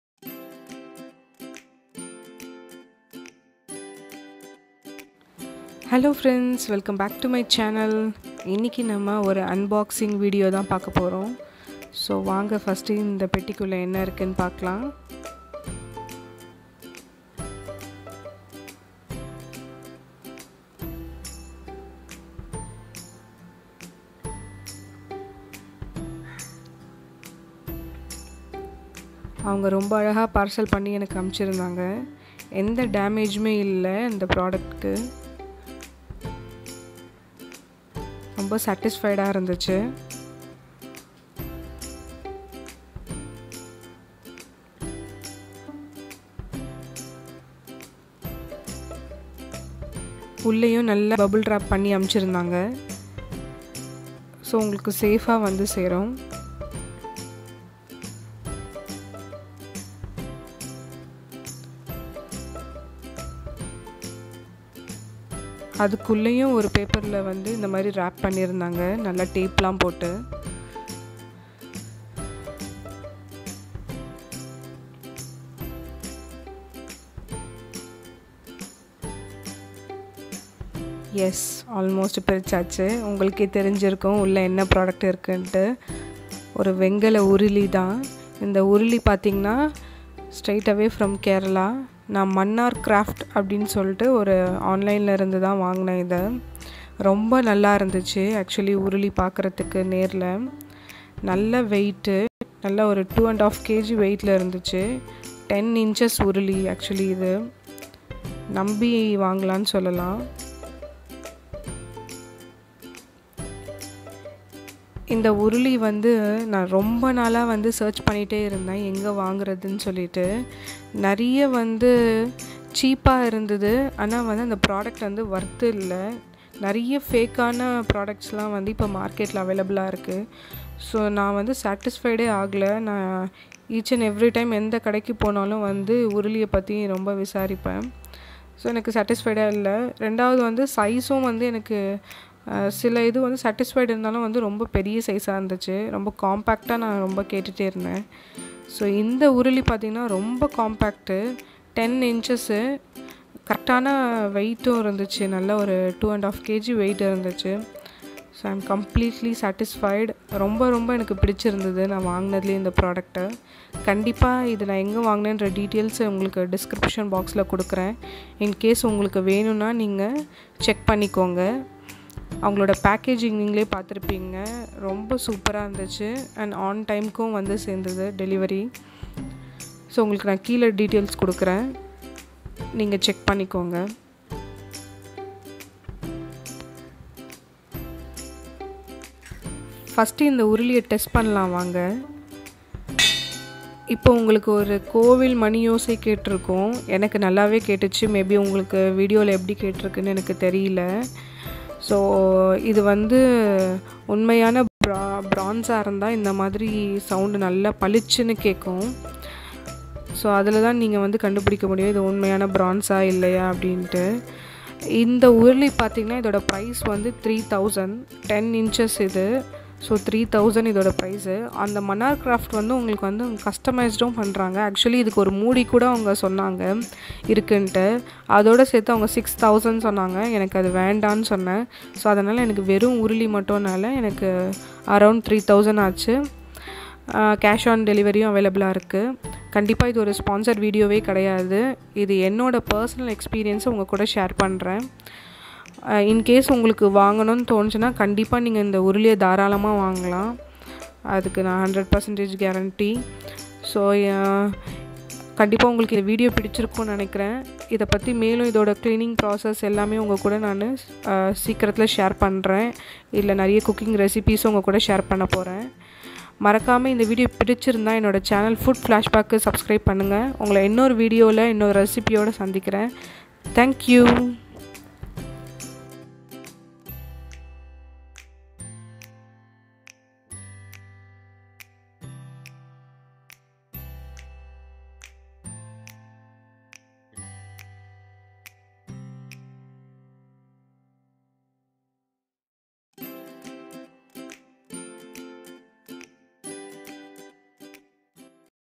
Unboxing bronze utensil | Vengala uruli| உருளி | unboxing uruli in tamil|cookware shopping
Hello friends in this video i have shared an unboxing my uruli and few details and i also shared my reviews about uruli if u need more details about this uruli do comment below for any details.